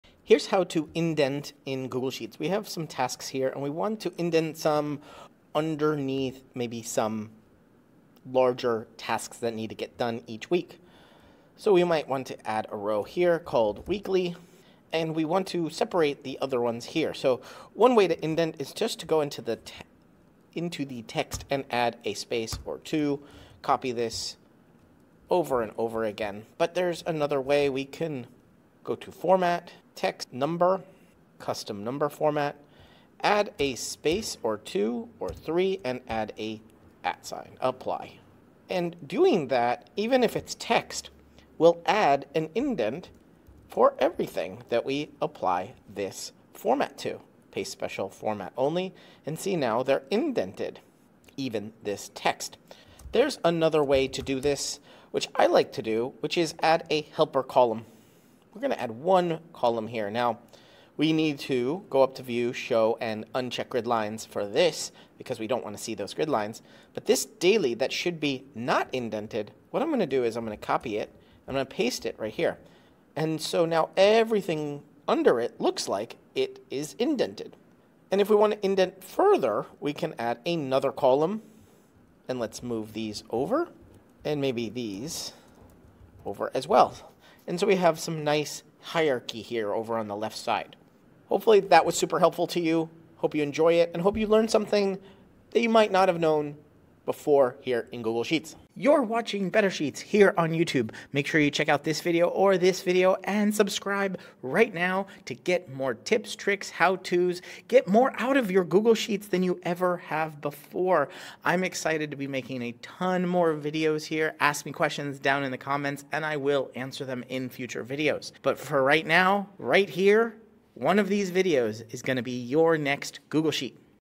How to Indent Google Sheets
How to Indent in Google Sheets: Easy Techniques

In this video, learn how to indent tasks in Google Sheets to create a more organized and visually appealing layout. We'll go over simple methods like adding spaces, using custom number formats, and utilizing helper columns to achieve a hierarchical structure. Follow along for step-by-step instructions and enhance your Google Sheets skills.

00:00 Introduction to Indenting in Google Sheets
00:17 Manual Indentation Method
00:26 Using Custom Number Format for Indentation
00:52 Indentation with Helper Columns
01:30 Final Tips and Encouragement
01:41 Subscribe for More Google Sheets Tips